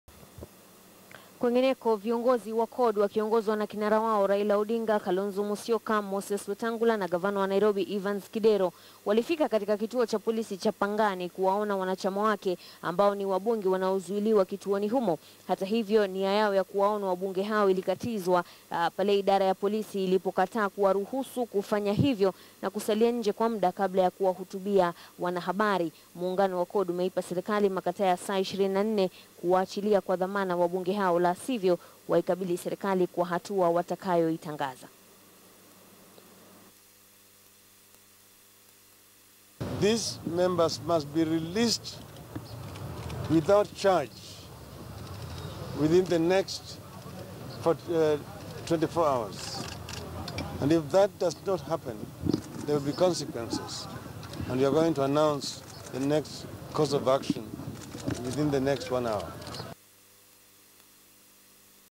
CORD yatoa matakaa ya saa 24
Viongozi wa CORD wakiongozwa na kinara wao Raila Odinga, Kalonzo Musyoka, Moses Wetangala na gavana wa Nairobi Evans Kidero, walifika katika kituo cha polisi cha Pangani kuwaona wanachama wake ambao ni wabunge wanaozuiliwa kituoni humo. Hatahivyo,nia yao ya kuwaona wabunge hao ilikatizwa pale idara ya polisi kutowaruhusu kufanya hivyo na kusalia nje kwa mda kabla ya kuhutubia vyombo vya habari. Muungano wa CORD umeipa serikali makataa ya saa 24 kuwaachilia kwa dhamana wabunge hao la sivyo waikabili serikali kwa hatua watakayoitangaza.